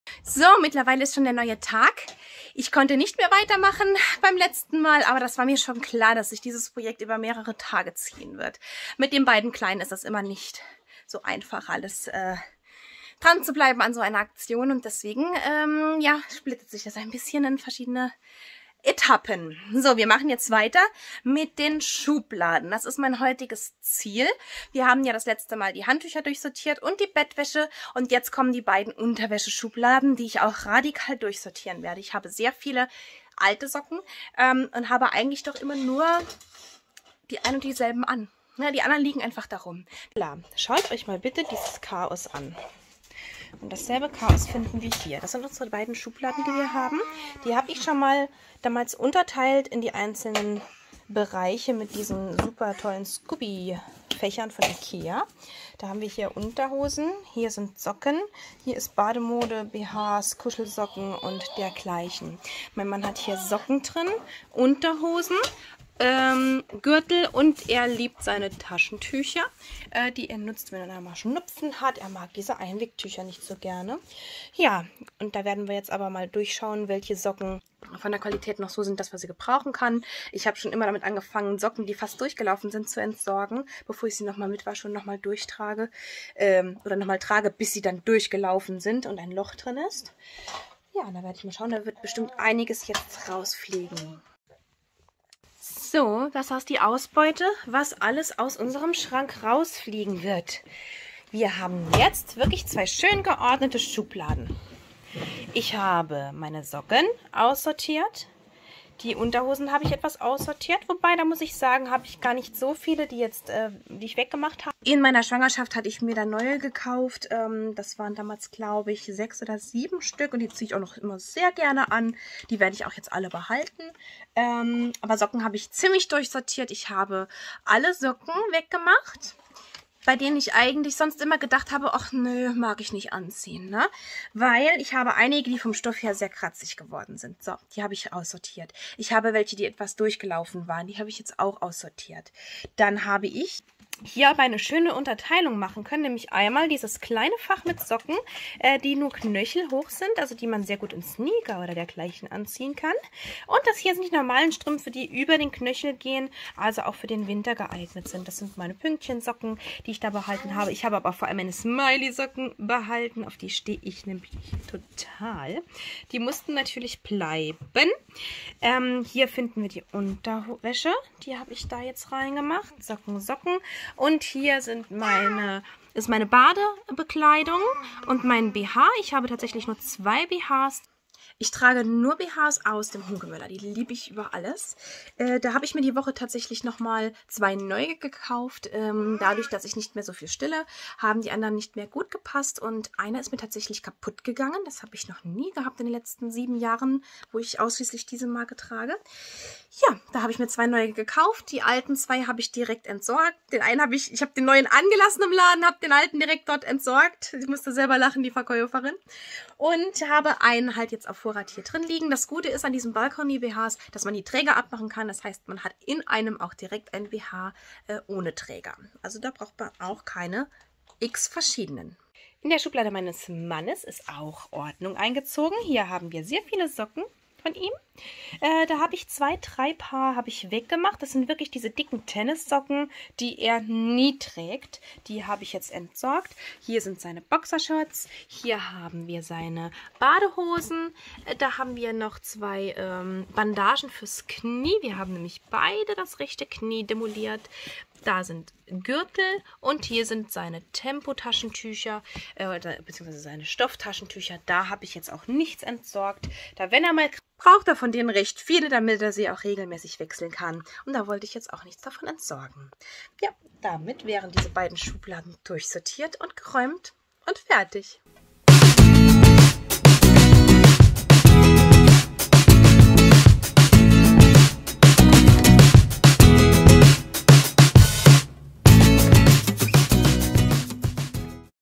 Nun sind die Schubladen dran! Minimalismus im Familienalltag
Willkommen zu meiner neuen Serie "Minimalismus im Familienalltag!" Gemeinsam werden wir unser Haus entrümpeln! Ich freue mich auf den Weg mit Euch zusammen!

Heute nehmen wir uns die Schubladenelemente vor und ich bin gespannt auf das Vorher-Nacher-Ergebnis!

Viel Spaß beim Schauen und bis bald!

Eure Christina